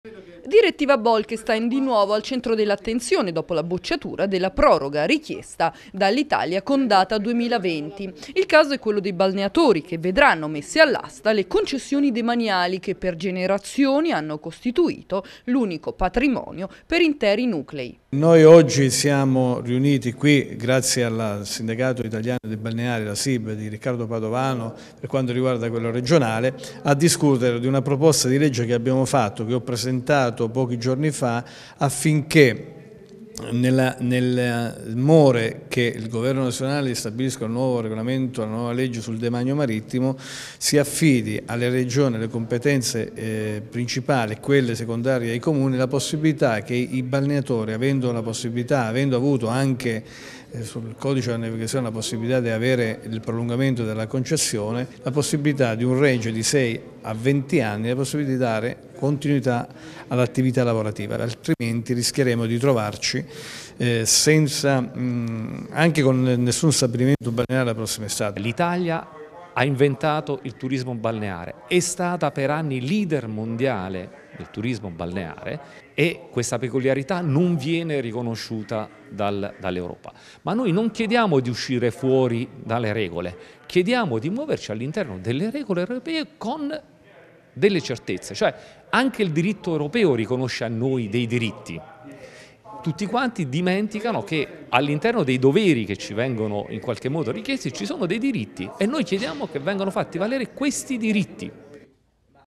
Bolkestein. L'Abruzzo chiede nuovo intervento al governo
Concessioni demaniali all'asta, l'introduzione della direttiva Bolkestein, tanto temuta dai balneatori italiani non potrà essere prorogata al 2020 come richiesto, dopo tanti appelli dalle istituzioni e dai balneatori italiani. Questa mattina a Pescara associazioni di categoria e rappresentanti della politica si sono riuniti per chiedere un intervento da parte del governo.